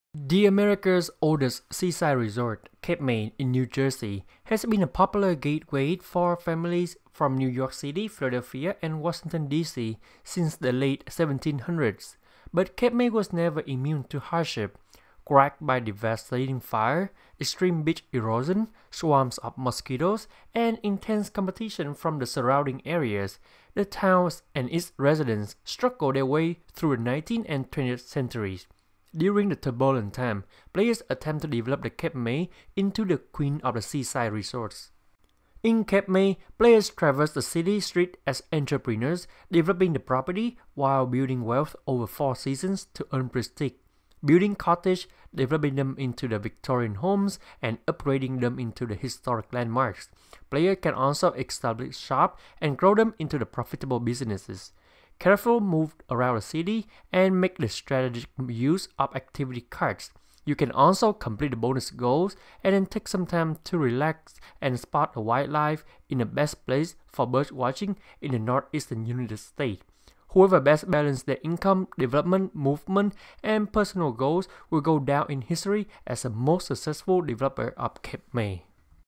Overview and How to Play Cape May Board Game
Pick up Cape May on Amazon

Cape May, the “Queen of the Seaside Resorts”, is located on the Southeastern tip of New Jersey and is one of the oldest seaside resorts in America. Founded in 1692 it boasts a beautiful collection of Victorian homes, churches and quaint shops.

In Cape May the board game, players travel the streets of this historic town, developing properties, avoiding catastrophes and spotting birds all the while earning prestige.

The player with the most prestige at the end of the game wins.

Follow our Amazon Influencer Store for more photos, vids and product recommends